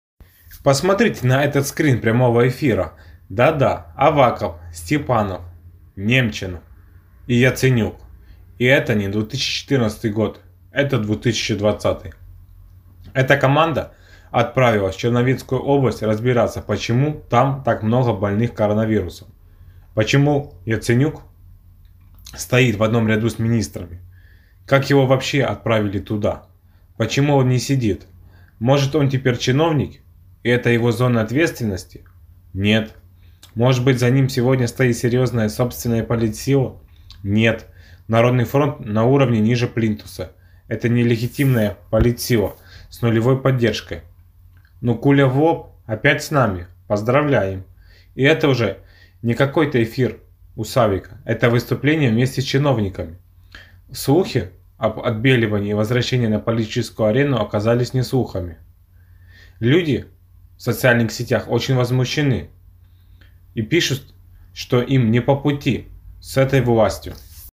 Посмотрите на этот скрин прямого эфира. Почему он не сидит?
Посмотрите на этот скрин прямого эфира. Да-да, Аваков, Степанов, Немчинов…и Яценюк. И это НЕ 2014 год. А 2020!